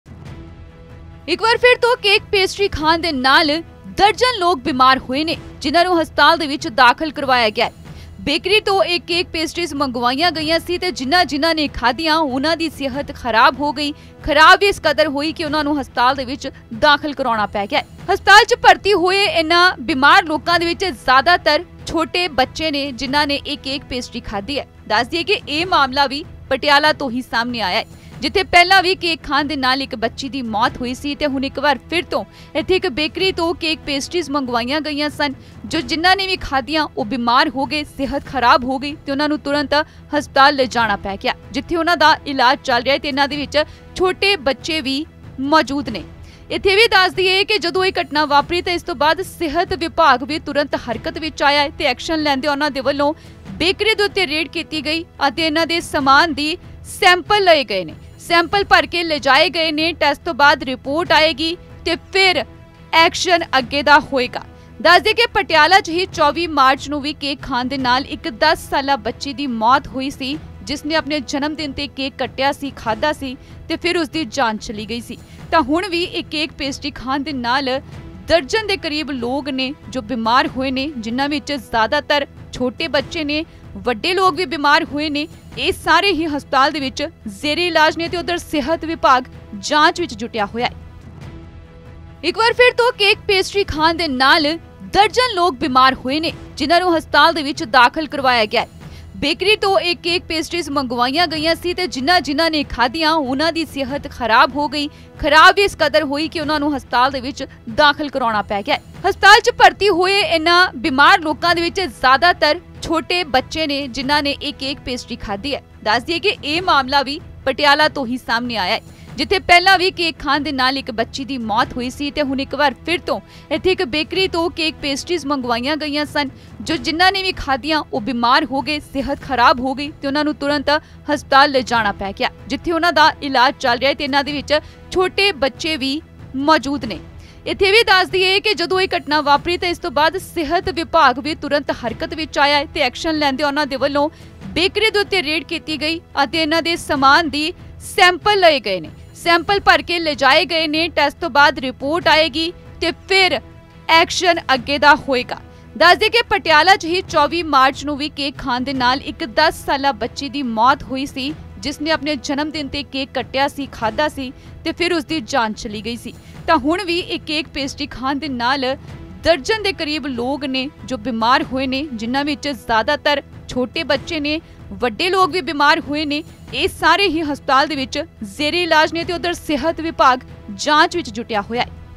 Patiala News | ਕੇਕ-ਪੇਸਟਰੀ ਖਾਣ ਨਾਲ ਬੱਚਿਆਂ ਸਣੇ ਦਰਜਨ ਲੋਕਾਂ ਦੀ ਵਿਗੜੀ ਸਿਹਤ
ਪਟਿਆਲਾ ਵਿੱਚ ਇੱਕ ਵਾਰ ਫਿਰ ਇੱਕ ਬੇਕਰੀ ਤੋਂ ਮੰਗਵਾਏ ਕੇਕ ਅਤੇ ਪੇਸਟਰੀ ਖਾਣ ਨਾਲ ਇੱਕ ਦਰਜਨ ਦੇ ਕਰੀਬ ਲੋਕਾਂ ਦੀ ਸਿਹਤ ਵਿਗੜ ਗਈ, ਜਿਨ੍ਹਾਂ ਵਿੱਚ ਜ਼ਿਆਦਾਤਰ ਬੱਚੇ ਹਨ, ਇਸ ਘਟਨਾ ਦਾ ਪਤਾ ਲੱਗਦਿਆਂ ਹੀ ਪਟਿਆਲਾ ਦੇ ਇੱਕ ਨਿੱਜੀ ਹਸਪਤਾਲ ਵਿੱਚ ਦਾਖਲ ਕਰਵਾਇਆ ਗਿਆ ਹੈ।